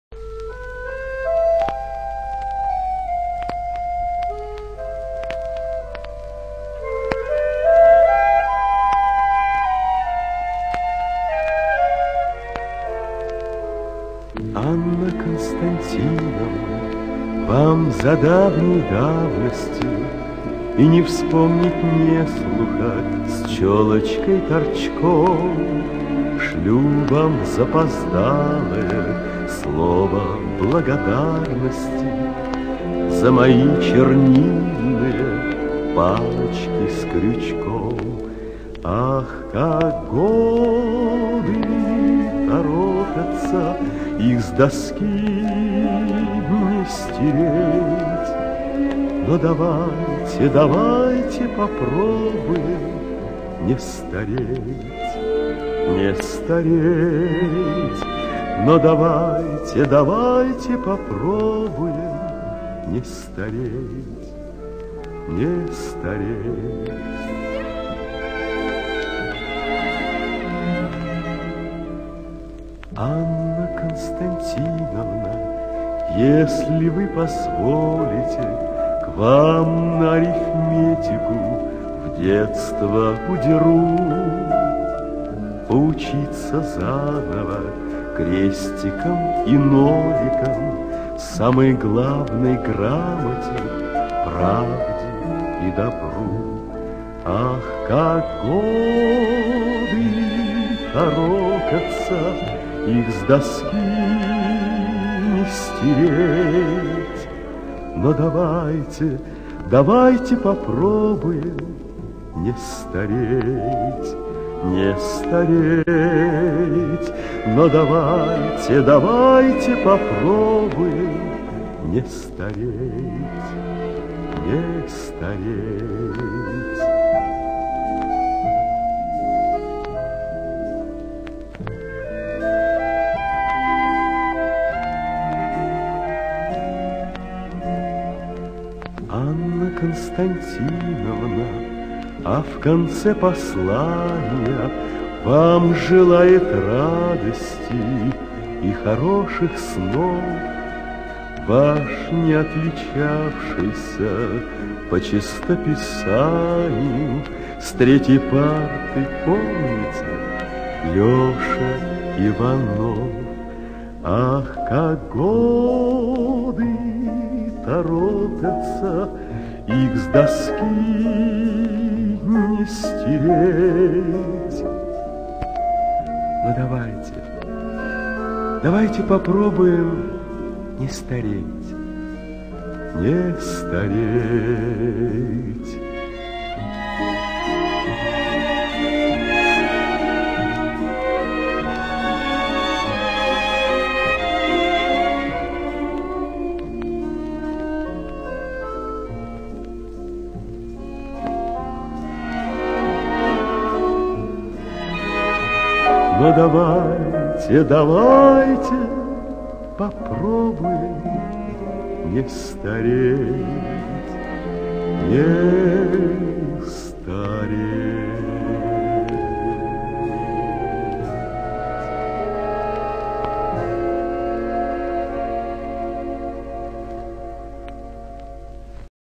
Посвящаю моей маме Нине Андреевне Солодовник, более 40 лет отдавшей благородной профессии Учителя .
Город Костанай, всем учителям краснопартизанской, надеждинской школы также посвящаю. К сожалению многих уже нет на этой земле. Но память о них сохраниться в сердцах их учеников!